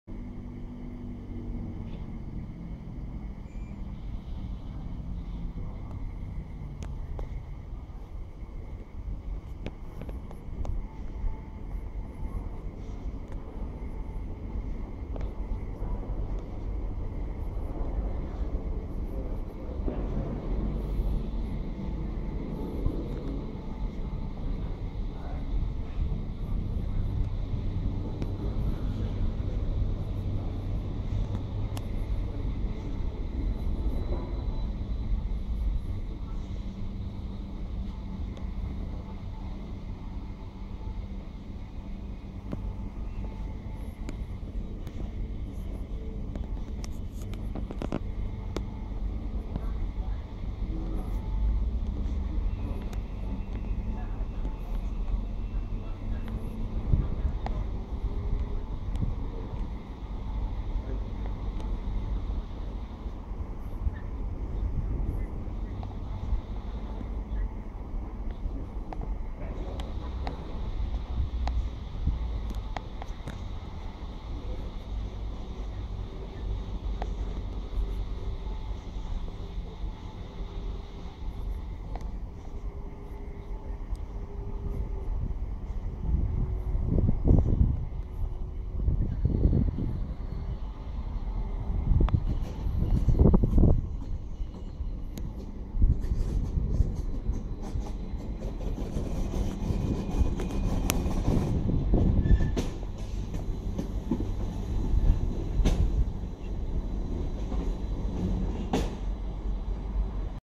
Mitcham Junction Train Station
Train Service From St Albans City To Sutton Calling At Mitcham Junction